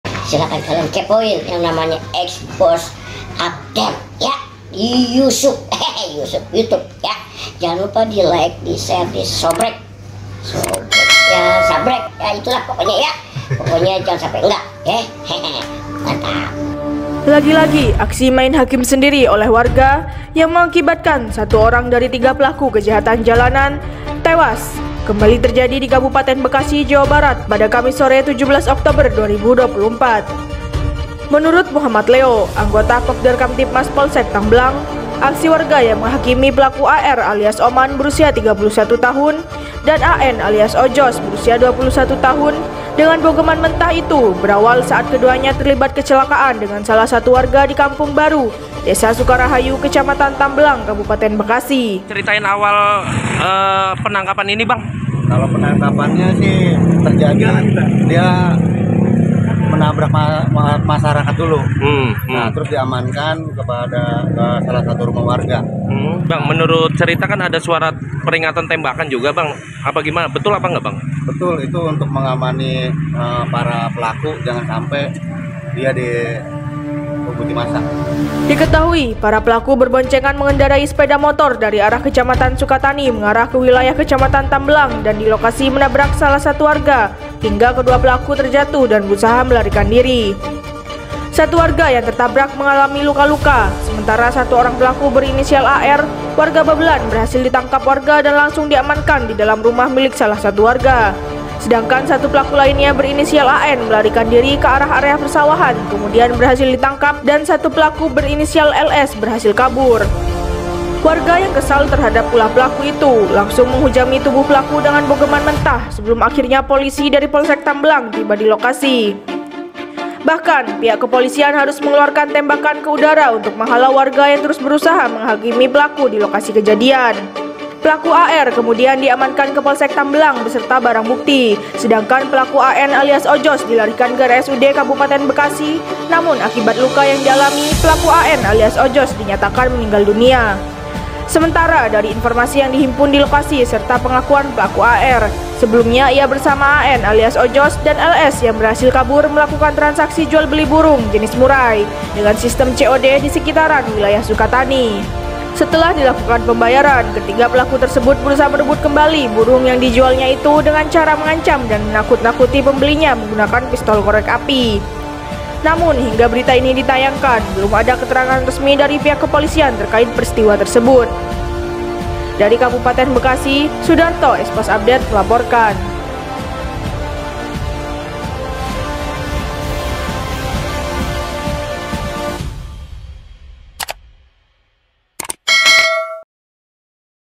SEMPAT TODONGKAN SENJATA, DIDUGA PELAKU PERAMPASAN MODUS JUAL BURUNG JADI BULAN BULAN WARGA
LAGI-LAGI AKSI MAIN HAKIM SENDIRI OLEH WARGA YANG MENGAKIBATKAN SATU ORANG DARI TIGA PELAKU KEJAHATAN JALANAN TEWAS KEMBALI TERJADI DI KABUPATEN BEKASI, JAWA BARAT, PADA KAMIS SORE, 17 OKTOBER 2024.

MENURUT MUHAMMAD LEO ANGGOTA POKDARKAMTIBMAS POLSEK TAMBELANG, AKSI WARGA YANG MENGHAKIMI PELAKU AR ALIAS OMAN (31) DAN AN ALIAS OJOS (21) DENGAN BOGEMAN MENTAH ITU BERAWAL SAAT KEDUANYA TERLIBAT KECELAKAAN DENGAN SALAH SATU WARGA DI KAMPUNG BARU, DESA SUKARAHAYU, KECAMATAN TAMBELANG, KABUPATEN BEKASI.

DIKETAHUI PARA PELAKU BERBONCENGAN MENGENDARAI SEPEDA MOTOR DARI ARAH KECAMATAN SUKATANI MENGARAH KE WILAYAH KECAMATAN TAMBELANG DAN DI LOKASI MENABRAK SALAH SATU WARGA HINGGA KEDUA PELAKU TERJATUH DAN BERUSAHA MELARIKAN DIRI.

SATU WARGA YANG TERTABRAK MENGALAMI LUKA-LUKA. SEMENTARA, SATU ORANG PELAKU BERINISIAL AR WARGA BABELAN BERHASIL DITANGKAP WARGA DAN LANGSUNG DIAMANKAN DIDALAM RUMAH MILIK SALAH SATU WARGA, SEDANGKAN SATU PELAKU LAINNYA BERINISIAL AN  MELARIKAN DIRI KE ARAH AREA PERSAWAHAN KEMUDIAN BERHASIL DITANGKAP DAN 1 PELAKU BERINISIAL LS BERHASIL KABUR.

WARGA YANG KESAL TERHADAP ULAH PELAKU ITU LANGSUNG MENGHUJANI TUBUH PELAKU DENGAN BOGEMAN MENTAH SEBELUM AKHIRNYA POLISI DARI POLSEK TAMBELANG TIBA DI LOKASI.

BAHKAN, PIHAK KEPOLISIAN HARUS MENGELUARKAN TEMBAKAN KE UDARA UNTUK MENGHALAU WARGA YANG TERUS BERUSAHA MENGHAKIMI PELAKU DI LOKASI KEJADIAN.

PELAKU AR KEMUDIAN DIAMANKAN KE POLSEK TAMBELANG BESERTA BARANG BUKTI, SEDANGKAN PELAKU AN ALIAS OJOS DILARIKAN KE RSUD KABUPATEN BEKASI. NAMUN AKIBAT LUKA YANG DIALAMI, PELAKU AN ALIAS OJOS DINYATAKAN MENINGGAL DUNIA.

SEMENTARA DARI INFORMASI YANG DIHIMPUN DI LOKASI SERTA PENGAKUAN PELAKU AR, SEBELUMNYA IA BERSAMA AN ALIAS OJOS, DAN LS YANG BERHASIL KABUR MELAKUKAN TRANSAKSI JUAL BELI BURUNG JENIS MURAI DENGAN SISTEM COD DI SEKITARAN WILAYAH SUKATANI.

SETELAH DILAKUKAN PEMBAYARAN, KETIGA PELAKU TERSEBUT BERUSAHA MEREBUT KEMBALI BURUNG YANG DIJUALNYA ITU DENGAN CARA MENGANCAM DAN MENAKUT-NAKUTI PEMBELINYA MENGGUNAKAN PISTOL KOREK API.

NAMUN HINGGA BERITA INI DITAYANGKAN, BELUM ADA KETERANGAN RESMI DARI PIHAK KEPOLISIAN TERKAIT PERISTIWA TERSEBUT.